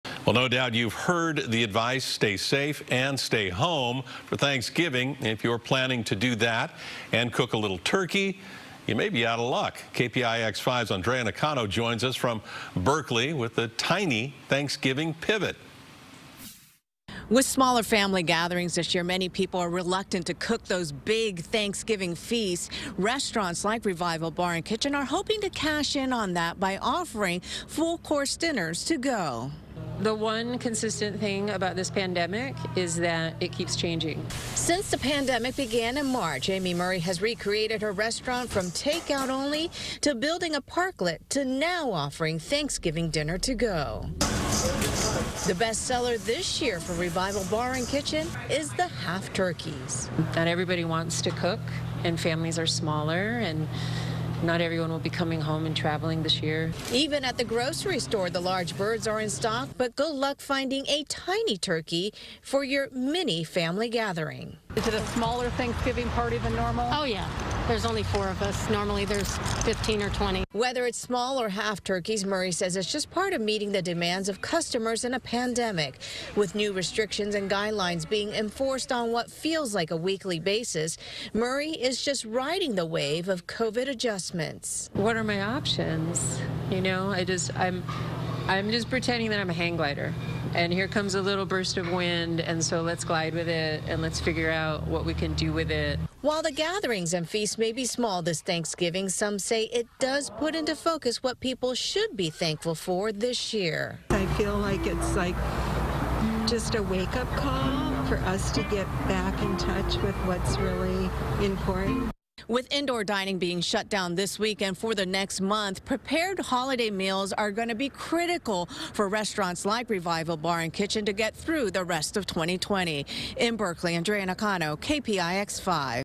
Pared-Back Pandemic Feasts Goose Demand for Tiny Turkeys
Across the Bay Area, small turkeys are almost sold out as families and the food industry adjust to downsized Thanksgiving plans during the pandemic. Andrea Nakano reports. (11-20-20)